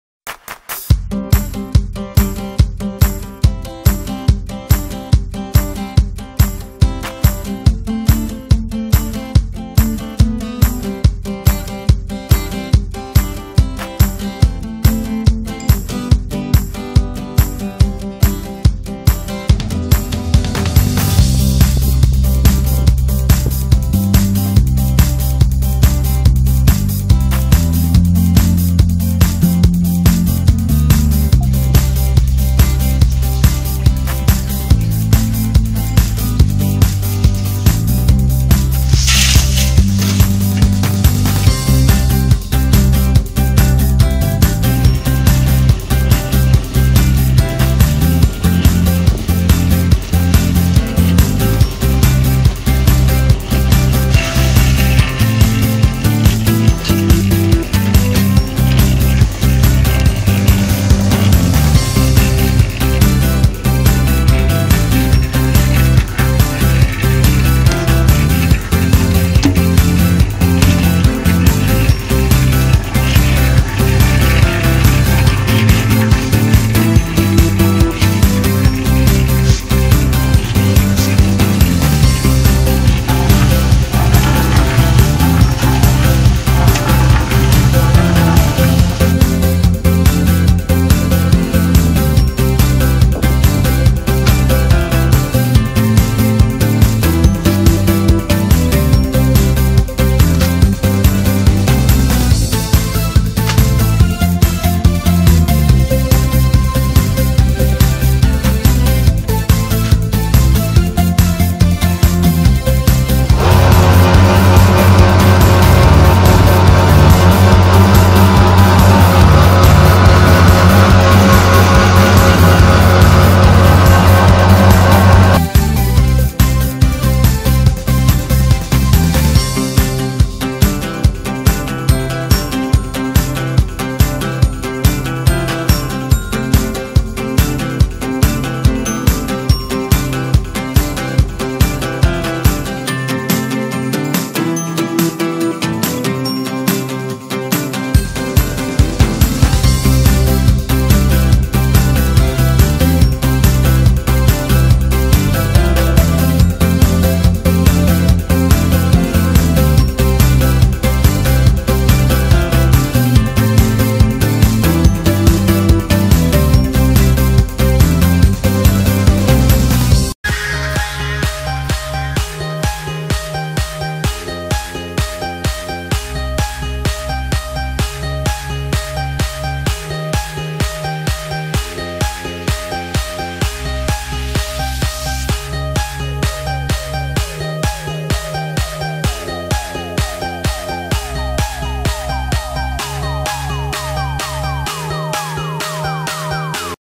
Производство тротуарной плитки в домашних условиях
Процес изготовления тротуарной плитки в домашних условиях с вибростолом !